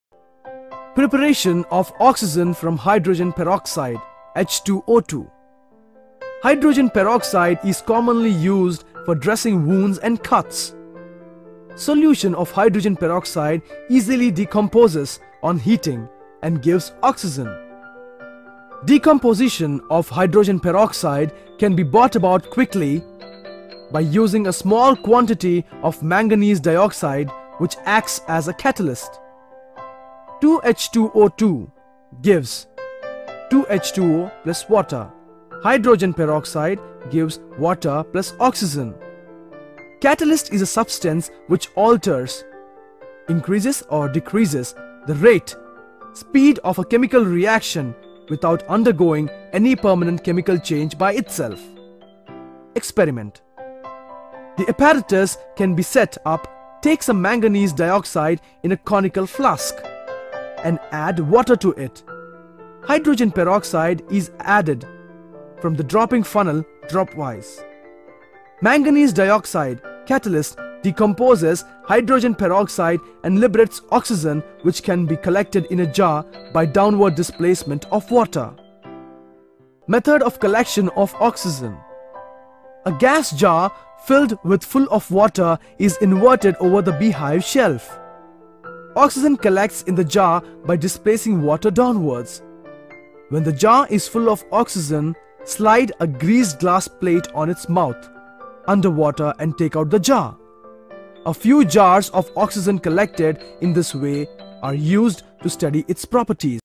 Preparation of Oxygen from Hydrogen Peroxide | 8th Class Science | Digital Teacher
Preparation of Oxygen from Hydrogen Peroxide. 8th Class Science. AP And Telangana State Syllabus Online by Digital Teacher.